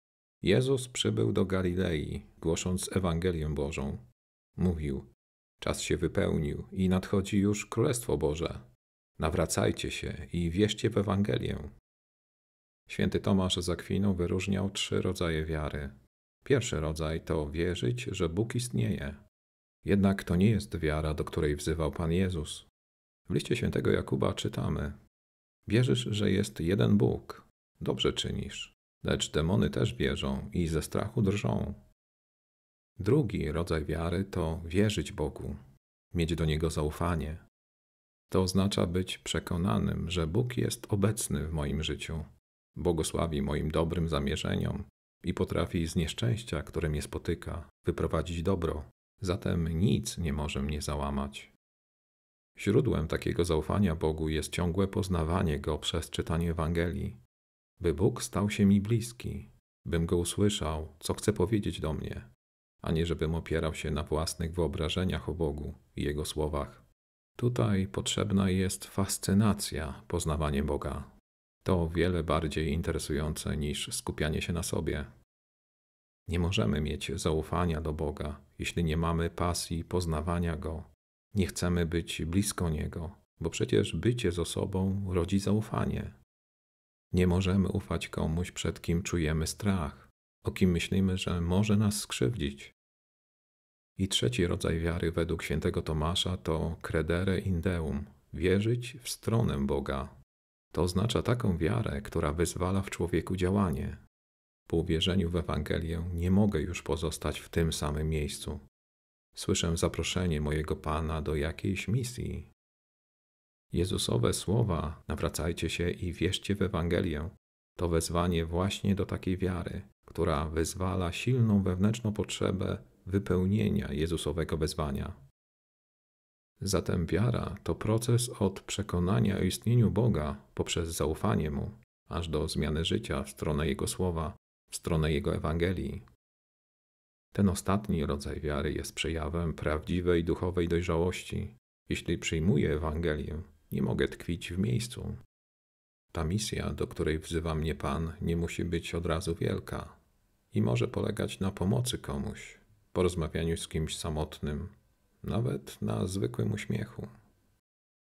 3 rodzaje wiary według św. Tomasza z Akwinu
Mk 1, 14–15. Jezus przybył do Galilei, głosząc Ewangelię Bożą. Mówił: „Czas się wypełnił” i „Nadchodzi już królestwo Boże! Nawracajcie się i wierzcie w Ewangelię”.